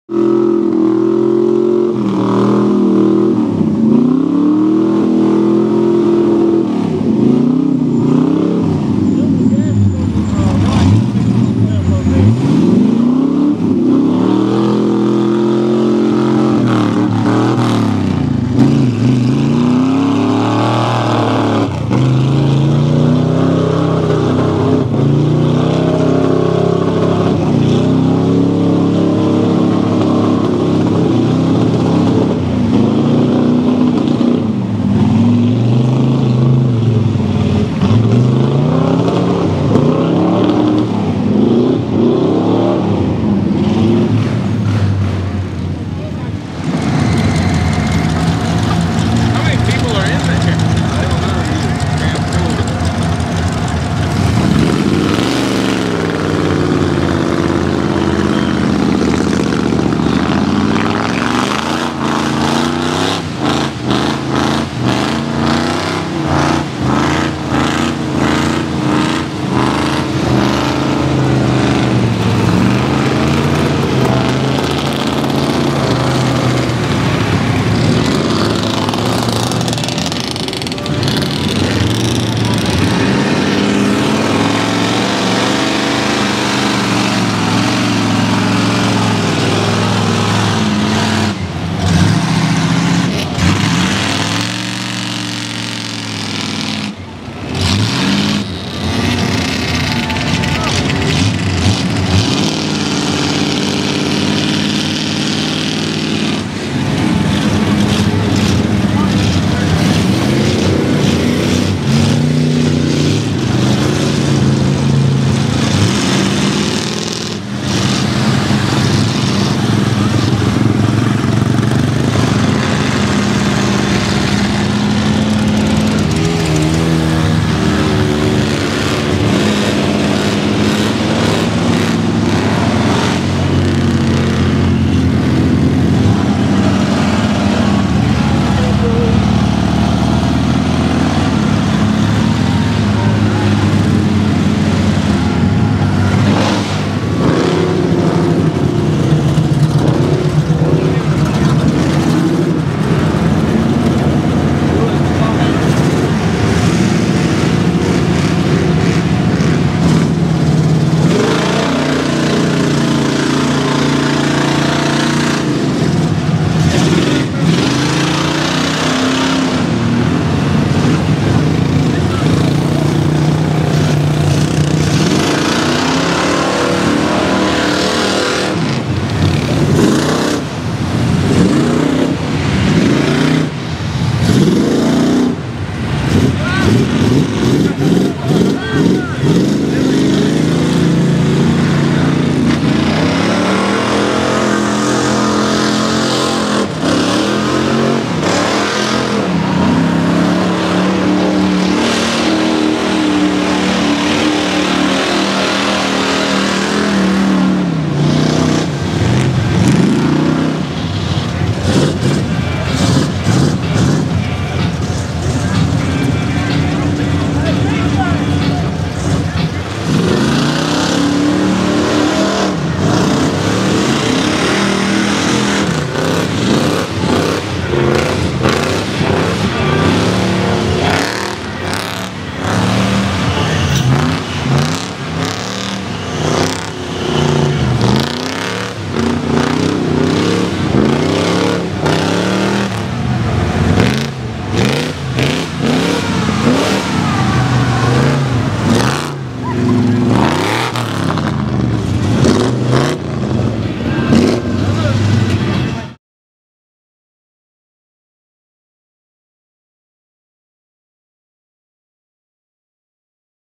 REDNECK MUD BOGGIN AT THE GAP
The Great American Park in Auburn Alabama has a couple of mud pits thats always attract a good time. Nothing like a Car on 44" Boggers playing in the mud.